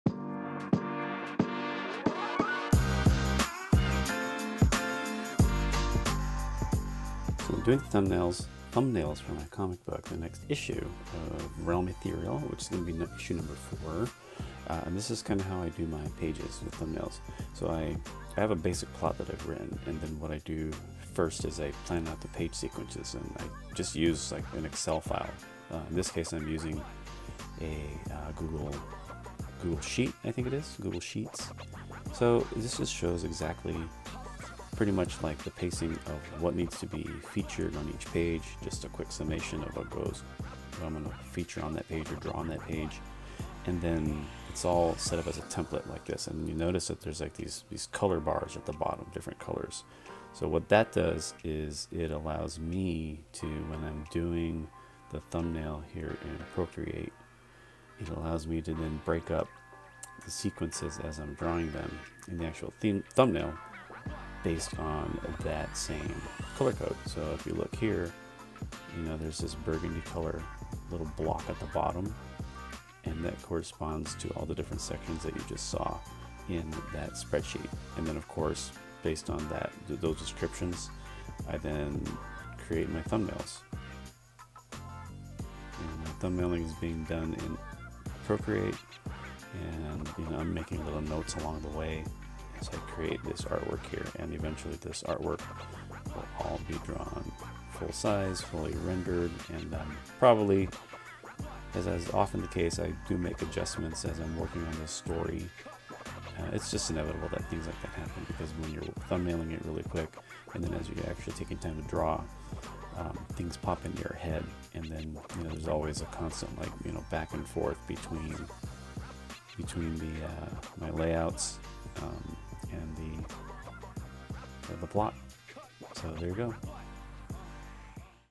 Creating Comic Book Thumbnails with Spreadsheets and Procreate
Every comic book page for my story is thumbnailed out before I start drawing the actual comic art. Making comic book thumbnails is an essential part of my working process when I create my comic book stories.

I also like to use a spreadsheet to help plan my pages, going from page to page. In this video, I'm using Google Sheets to make notes on what I'm drawing from page to page. The spreadsheets let me plan out the transitions from one sequence to the next.

I use Procreate to then draw out the thumbnails. Learn more about my thumbnailing process and the five reasons why you should make comic book thumbnails before drawing your comic book pages